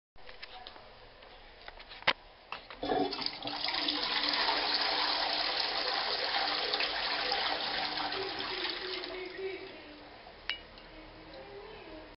Right cubicle, public loos, older twyfords
This is another original cubicl, i remember they all used to be original, but the middle got replaced. NIce flush.

P.s That's only cleaning stuff in the bowl